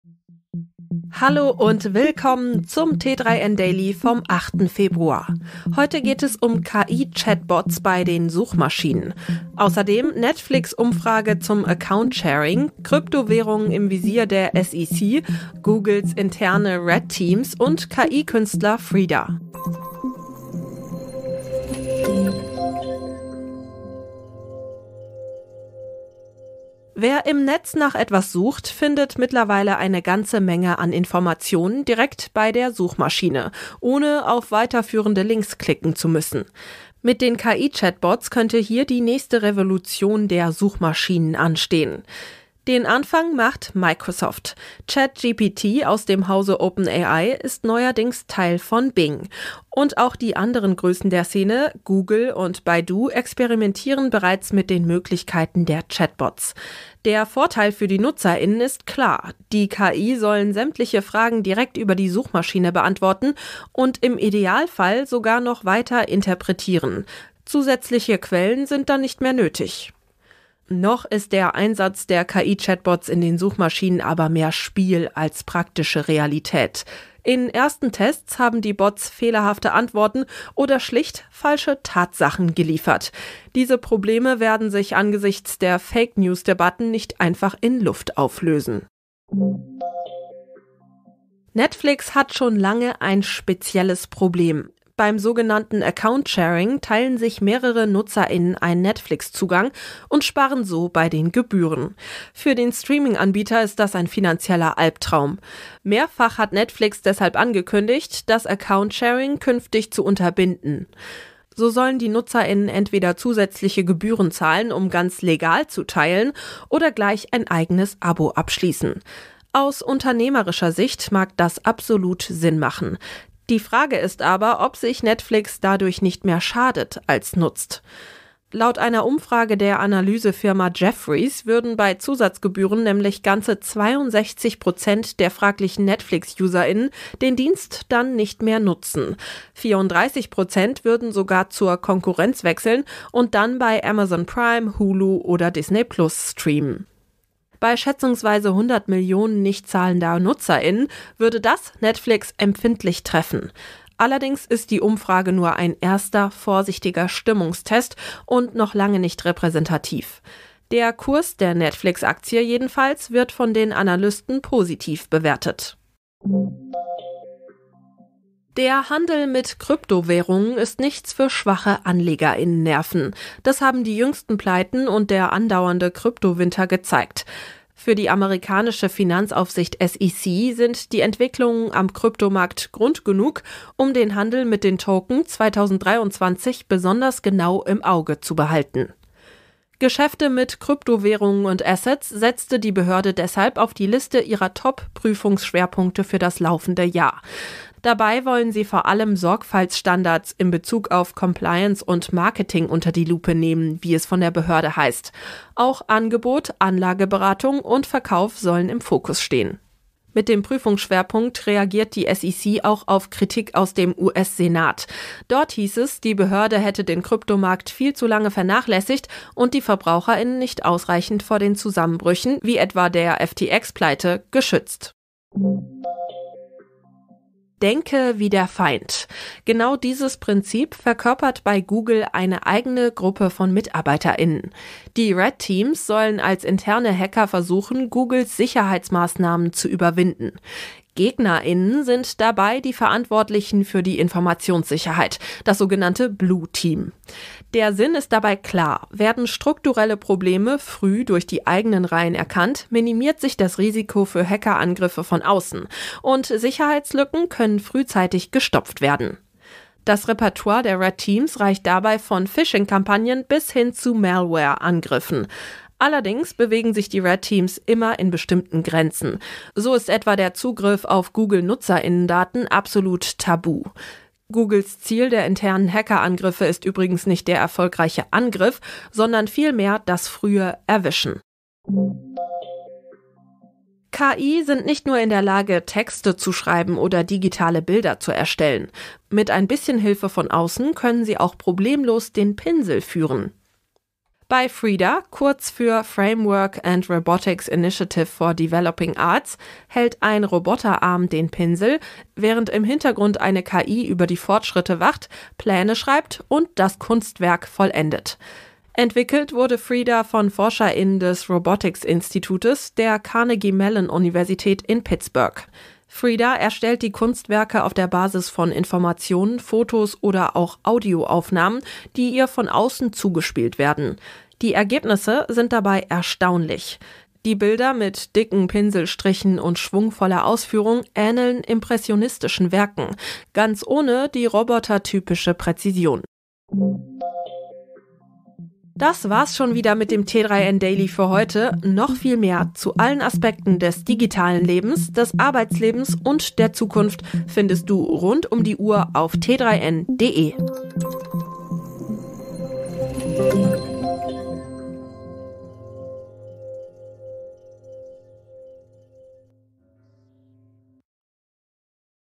Suche trifft KI: So wird sich die Websuche verändern - t3n Daily
Hallo und willkommen zum t3n Daily vom 8. Februar. Heute geht es um KI-Chatbots bei den Suchmaschinen. Außerdem: Netflix-Umfrage zum Account-Sharing, Kryptowährungen im Visier der SEC, Googles interne Red Teams und KI-Künstler Frida.